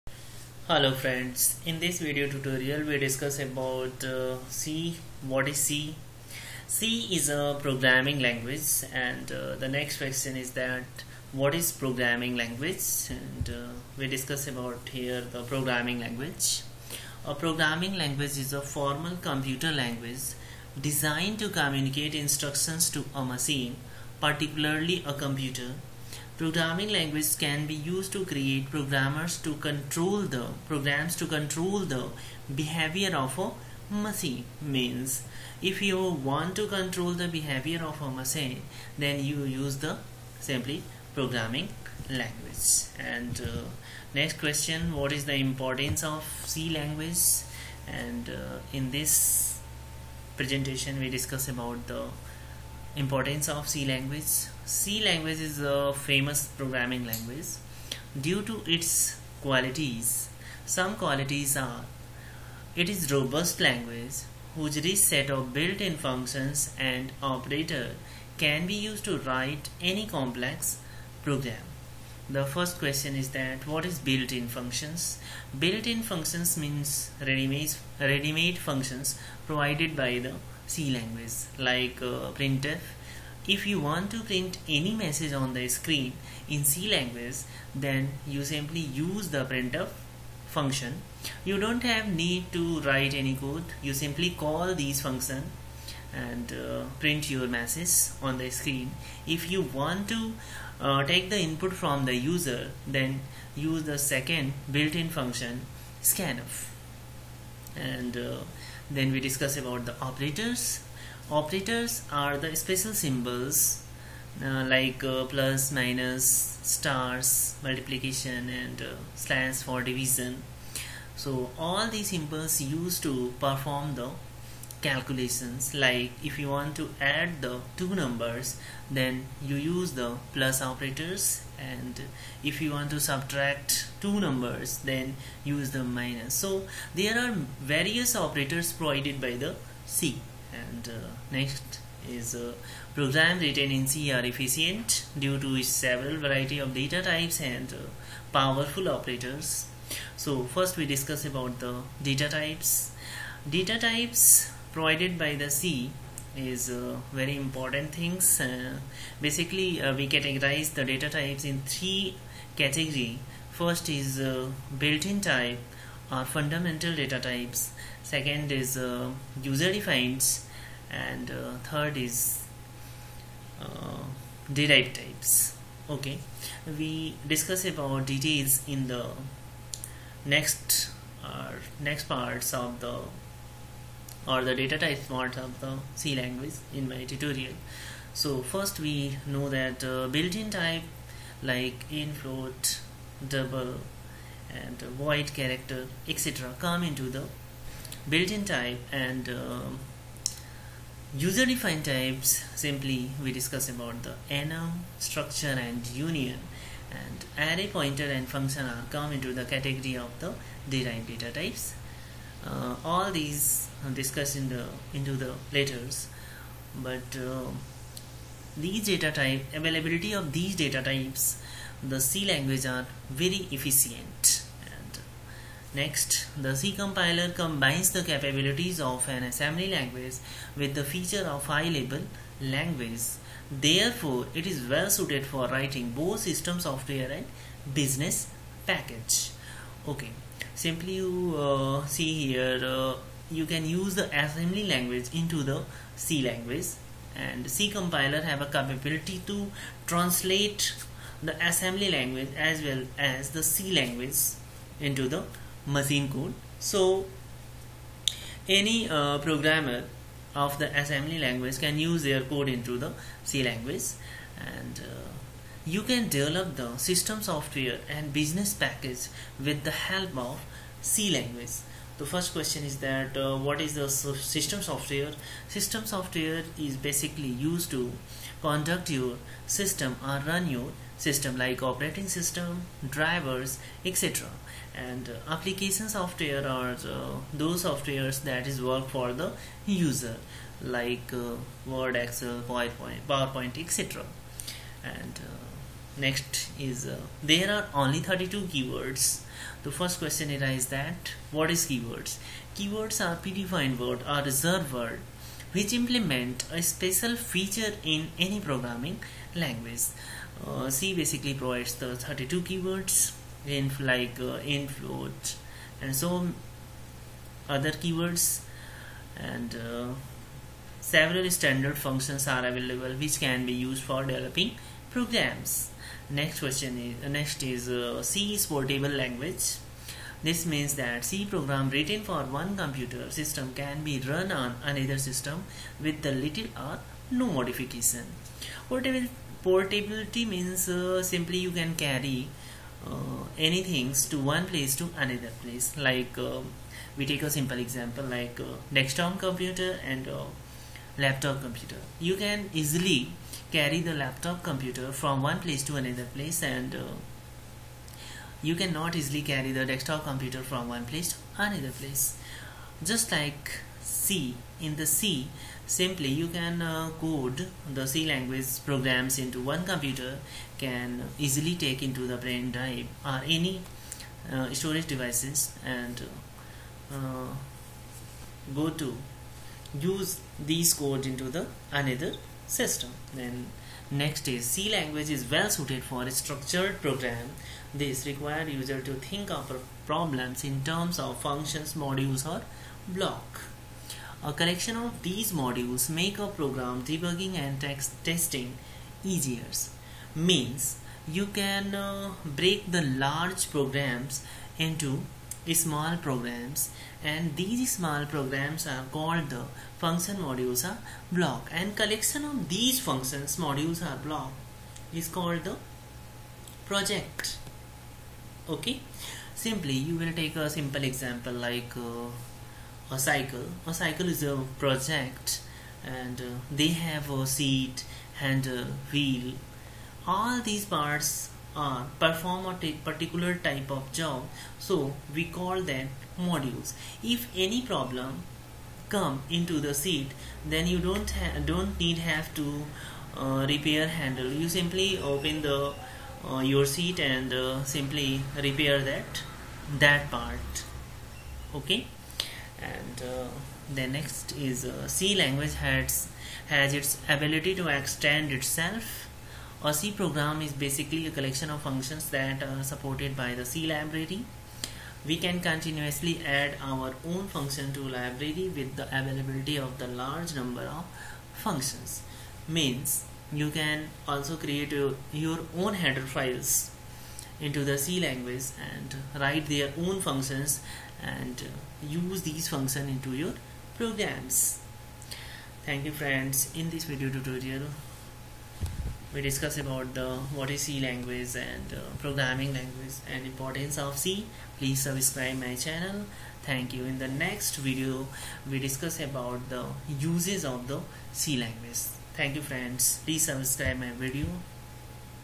What Is C Language and their importance # C Language # C-Tutorial # What is C # What Is C Language
In this video tutorials we discuss about what is c language and their importance